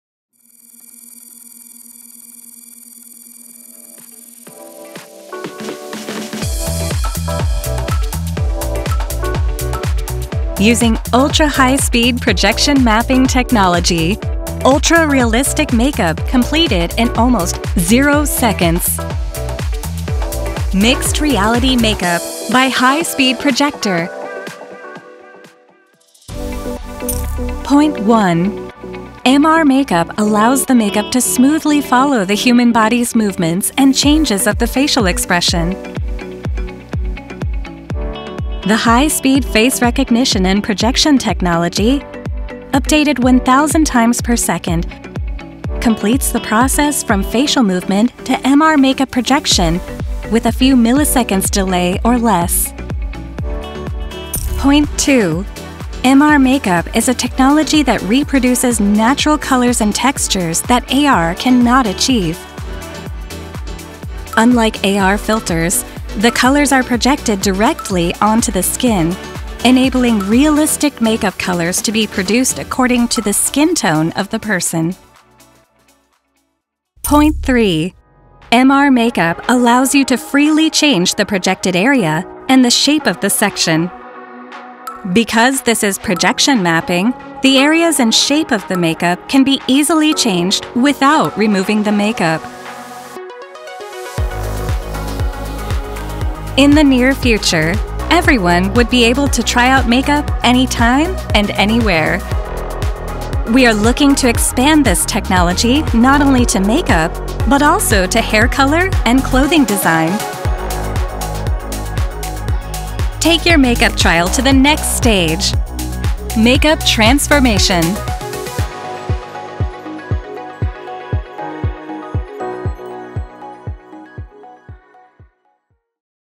CES2023 コーセー Mixed Reality Makeup
世界最大級のテクノロジー見本市CESにて、高速プロジェクションマッピング技術を活用したMixed Reality(複合現実)なメイク体験を提供しました。
▼詳しくはこちら（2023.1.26 ニュースリリース）